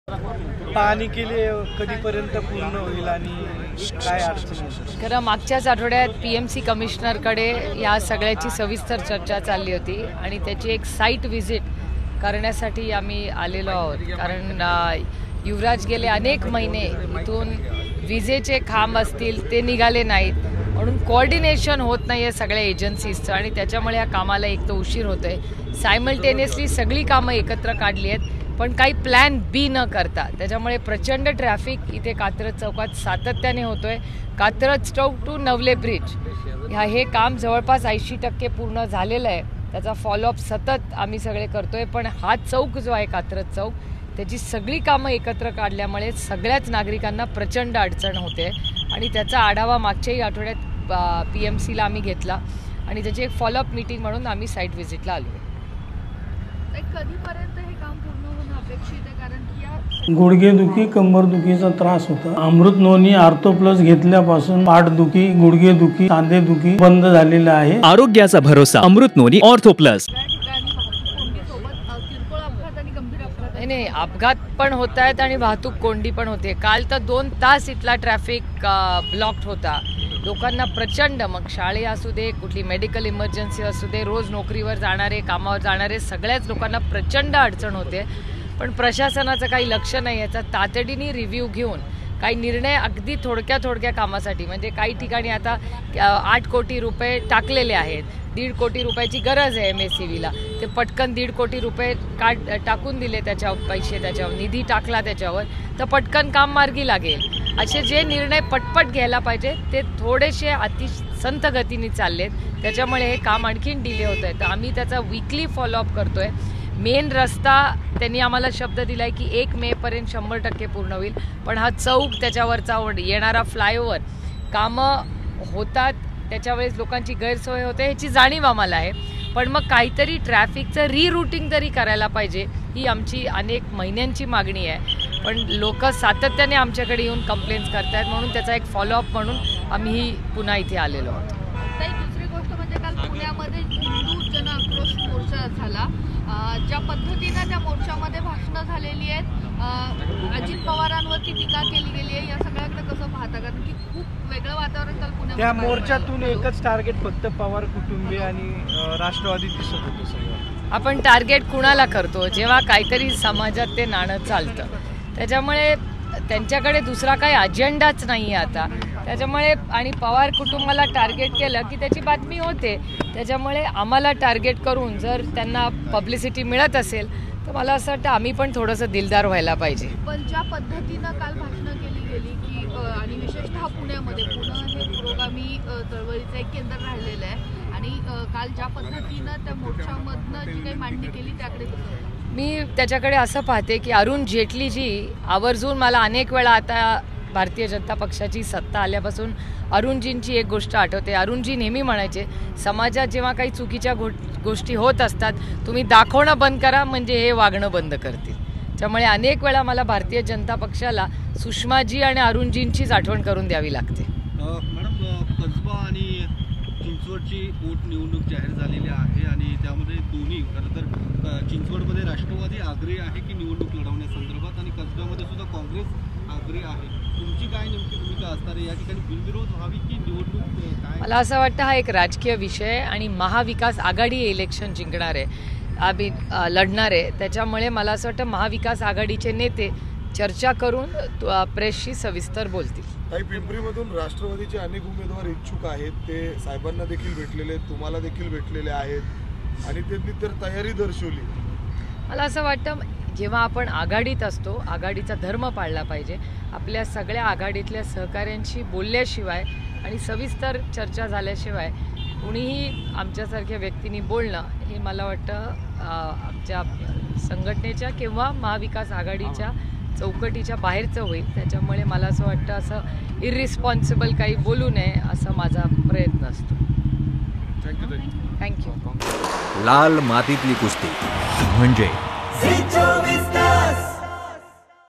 Supriya Sule | Kasaba and Chinchwad bypoll Election | NCP | MVA
Joint discomfort, back pain, neck and shoulder discomfort, or muscle stiffness and soreness can be treated with Amrith Noni Artho Plus.

Pune | NCP MP | Supriya Sule - Brief Media Uncut | 23 January 2023 N

Social Media Links :
5 Minitat 25 Batmya । 5 मिनिटांत 25 बातम्या